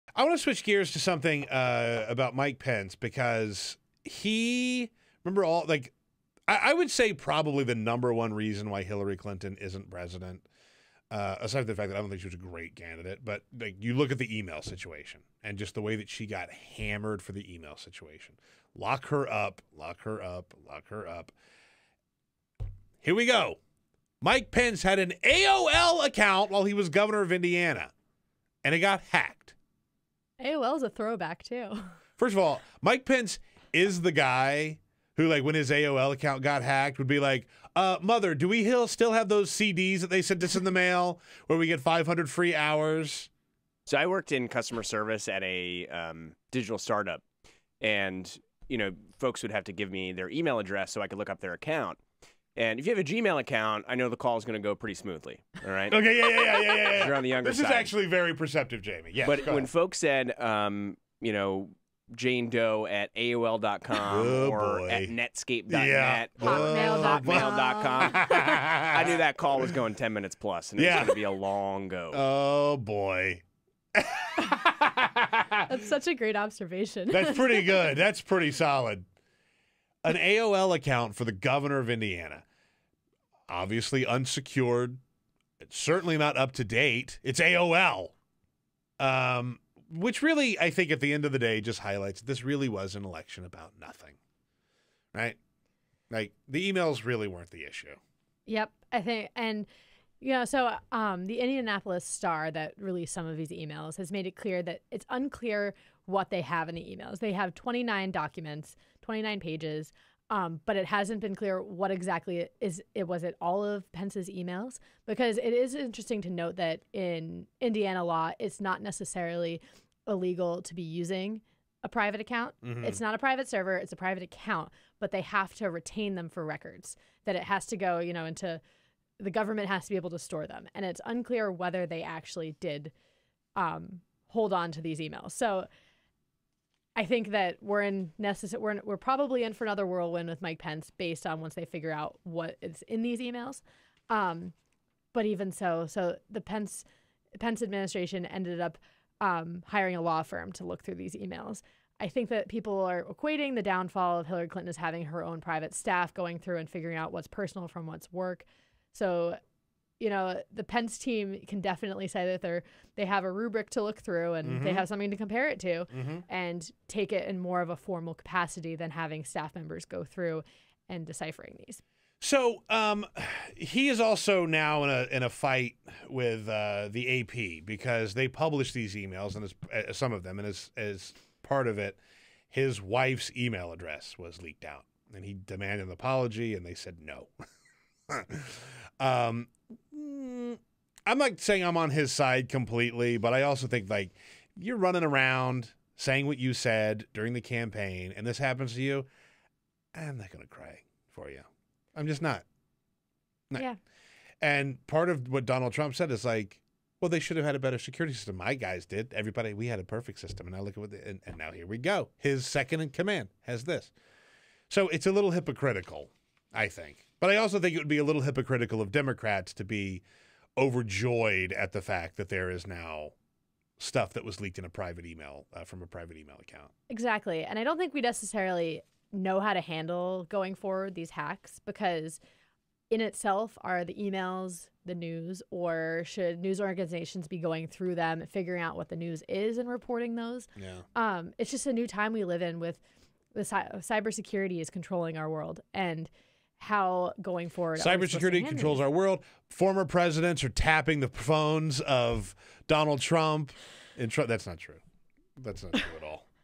Mike Pence's Private Email Account Got Hacked!
Vice President Mike Pence, former Governor of Indiana, used a private email account to conduct official business while in office. In a great turn of irony after Hillary Clinton's presidential bid was haunted by her private server,  Pence's emails are now being leaked from his hacked, private account. Peter Ogburn of TYT Network discusses with Cate Martell of The Hill.